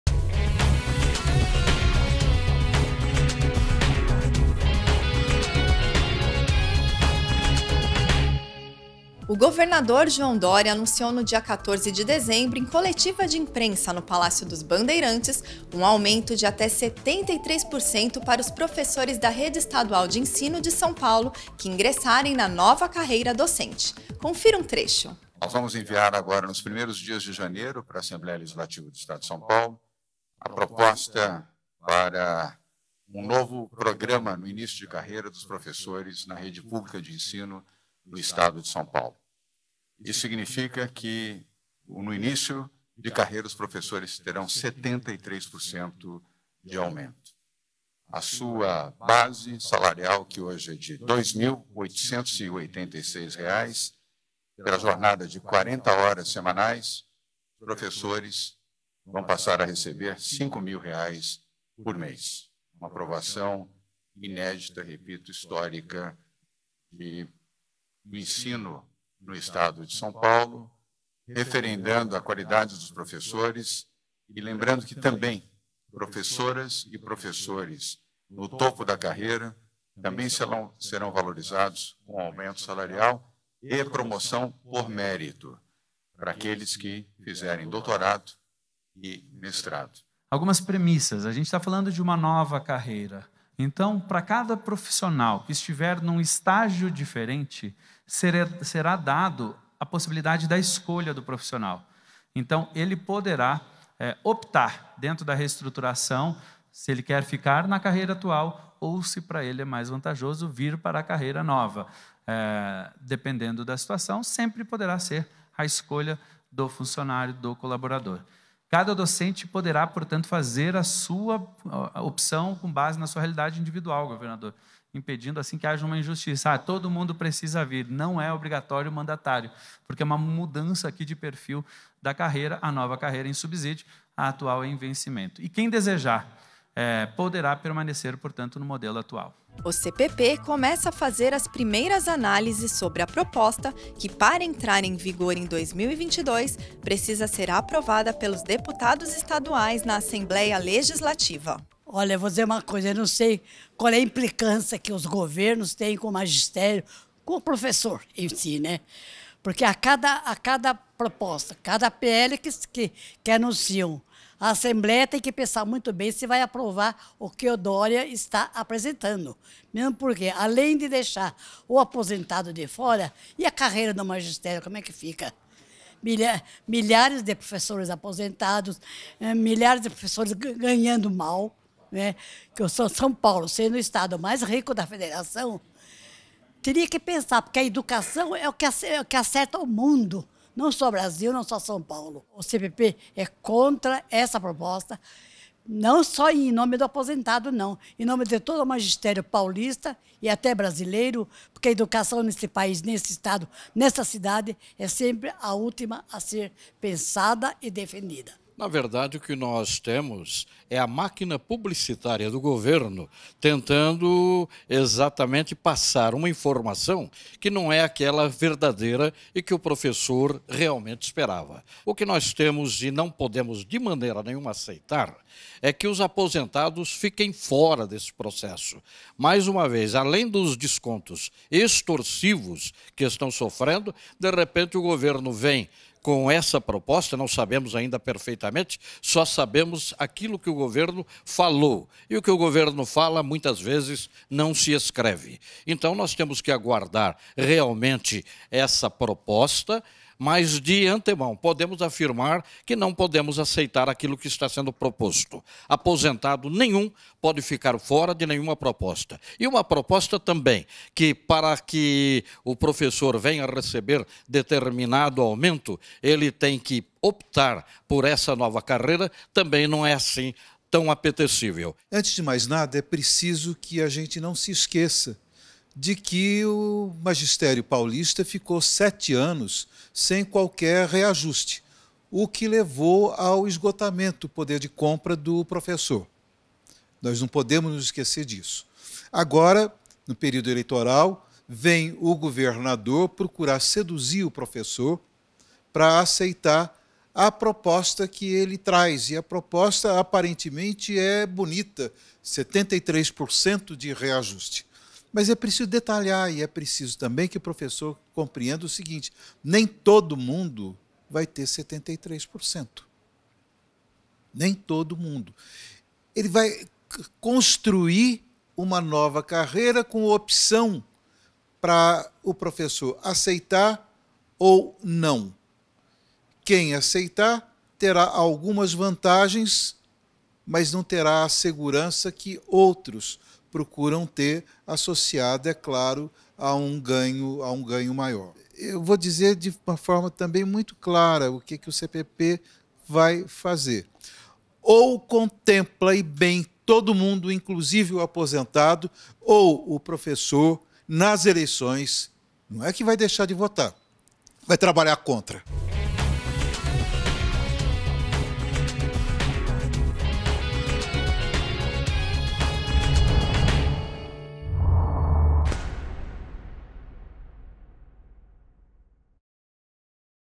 Professores podem ter aumento salarial de até 73% a partir de 2022 em SP
Governo de São Paulo anuncia plano de reajuste salarial para profissionais com jornadas de 40 horas semanais.